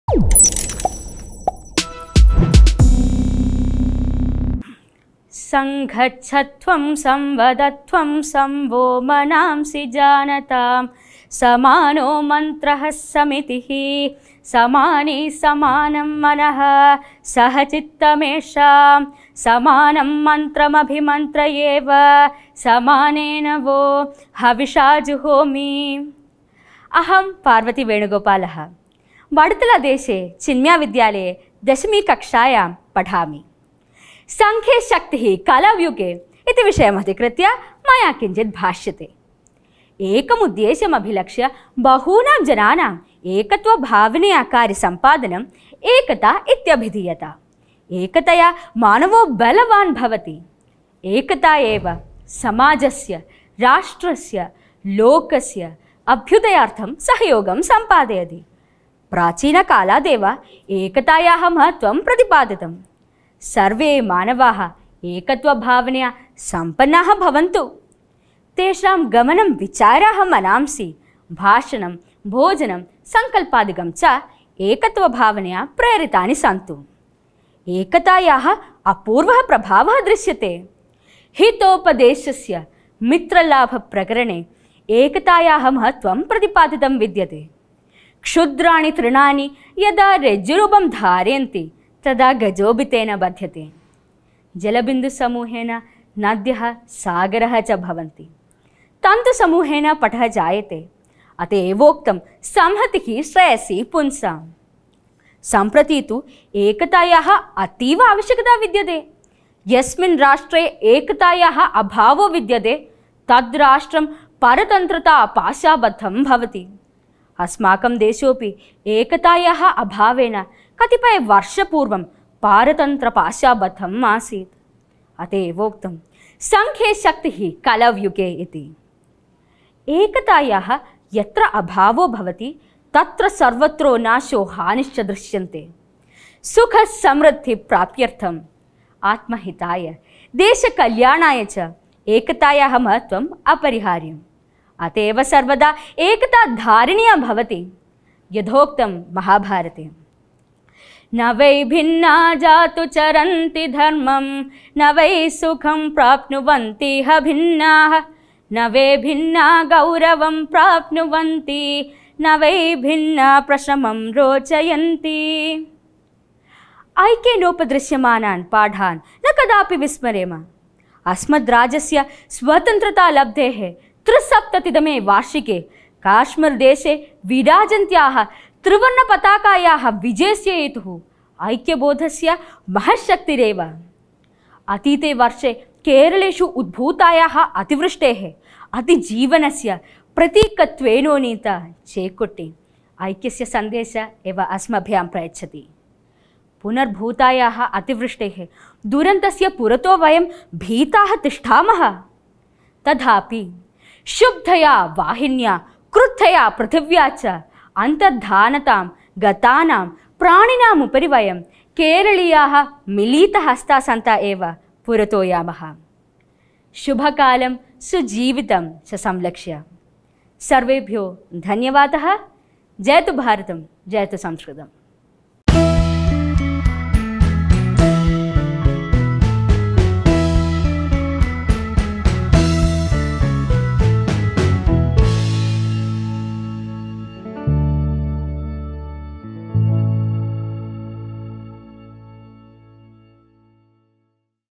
संघे शक्ति कलौ युगे | Sanskrit Speech by Parvathy Venugopal | Chinmaya Vidyalaya Vaduthala
PRIME TALKER: PARVATHY VENUGOPAL
SUBJECT: संघे शक्ति कलौ युगे
SCHOOL: CHINMAYA VIDYALAYA VADUTHALA